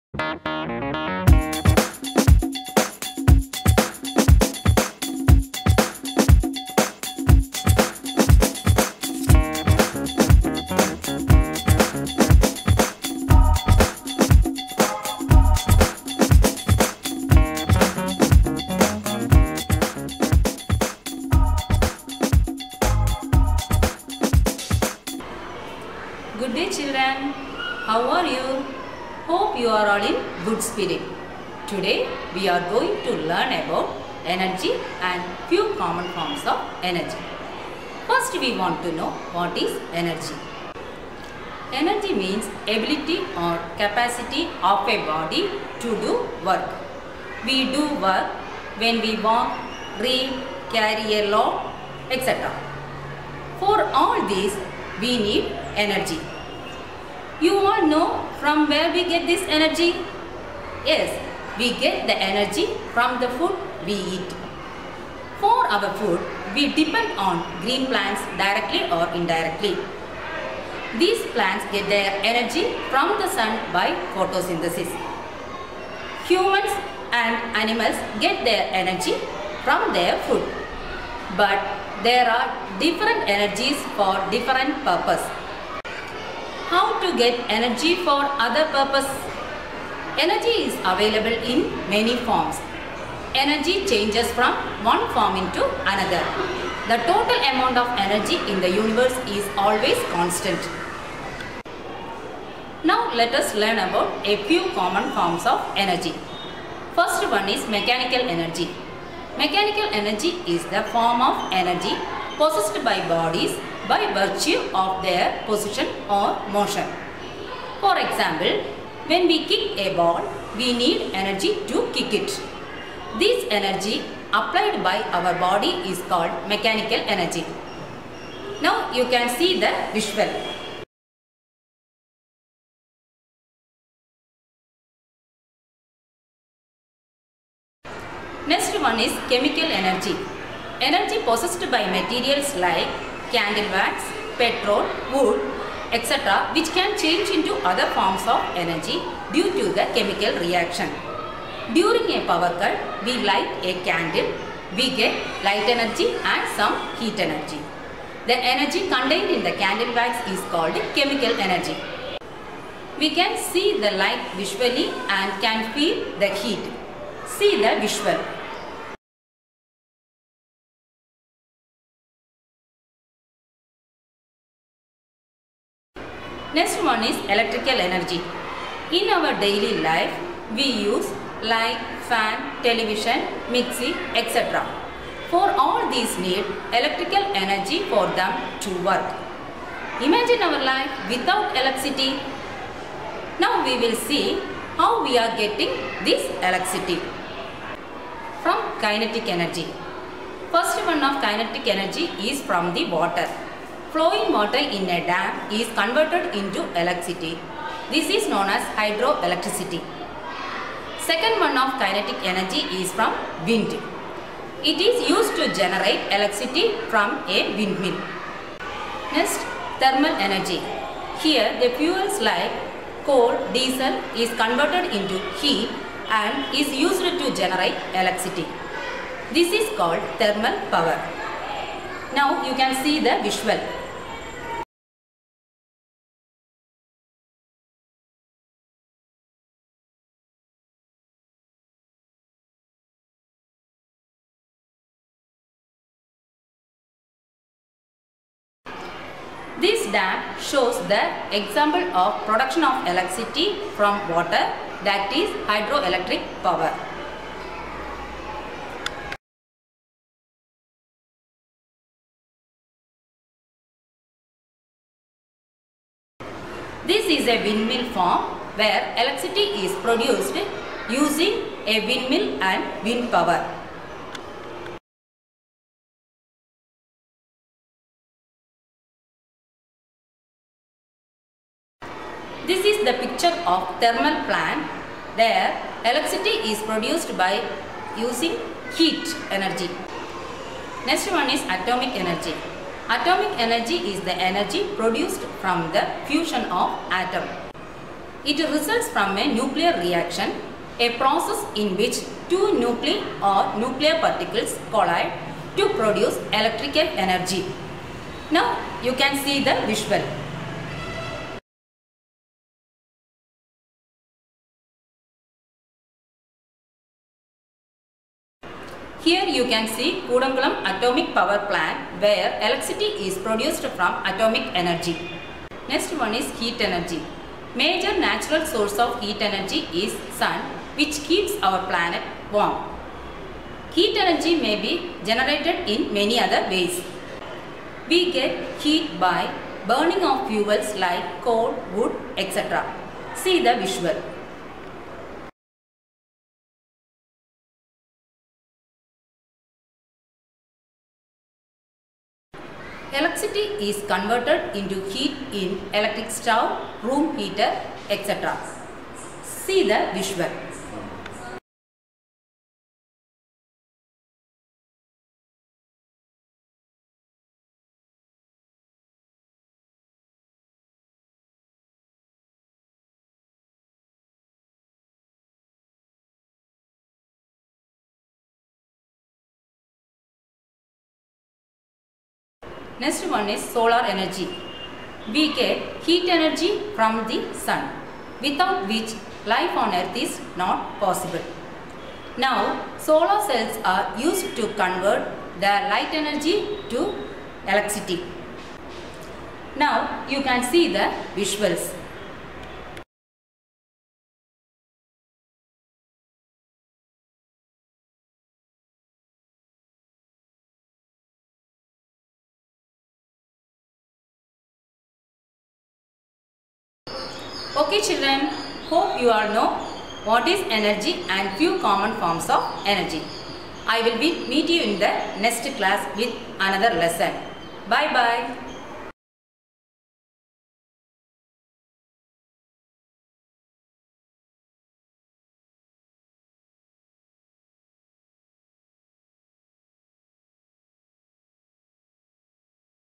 ENERGY by Mrs Mini
This skpschool video lesson is by our teacher Mrs.Mini. The topic is ENERGY and is explained with suitable visuals